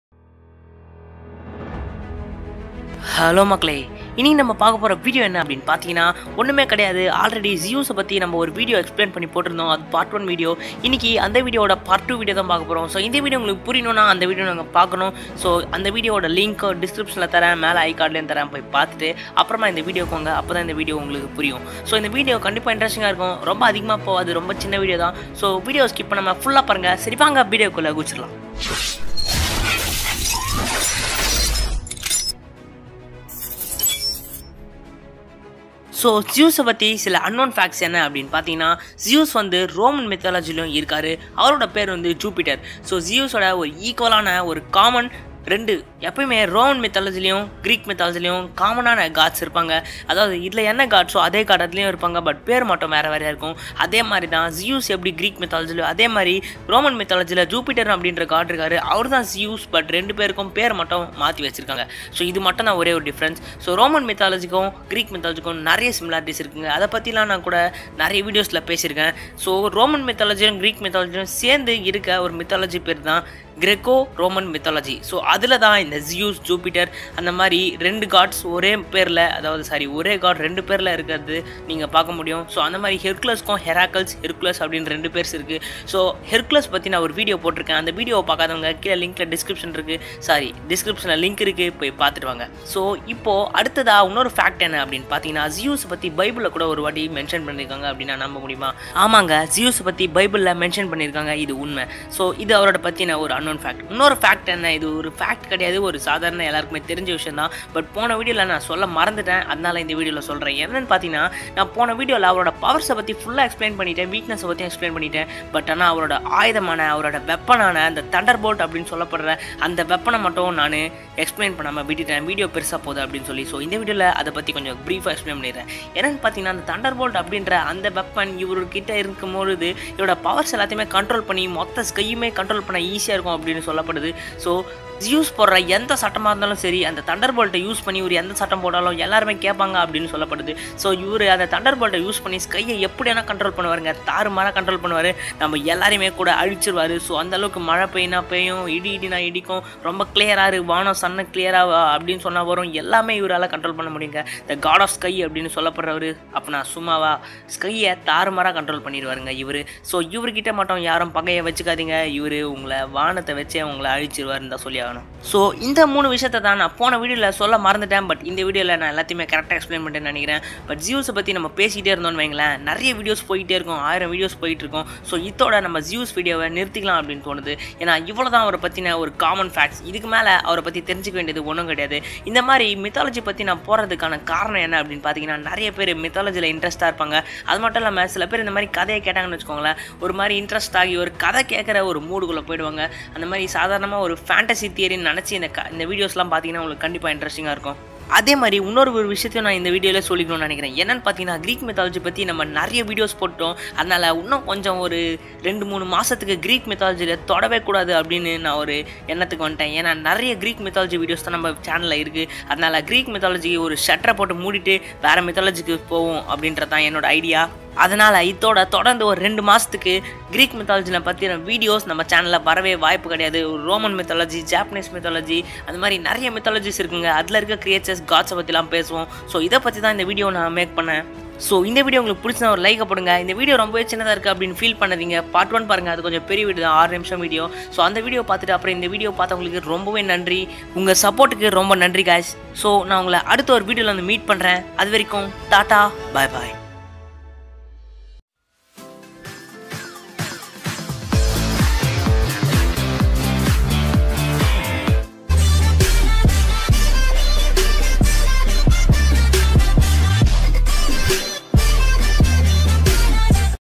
ZEUS Powers and Unknown Facts | Part 2 | dull mashup
Hello makkale,
       This is the video about the god of sky in Greek and roman mythology named Zeus in Greek and Jupiter in roman mythology. Part 1 video is explained already. Now its second part. Watch the first part  and then come here. Then you can understand this video better. His thunderbolt is his great weapon. He is the god of sky. With his thunderbolt he can control all over the sky. He is the god of all gods. I explained his unknown facts in Tamil. I hope you all like this video. Enjoy the video.

Hercules explanation video:-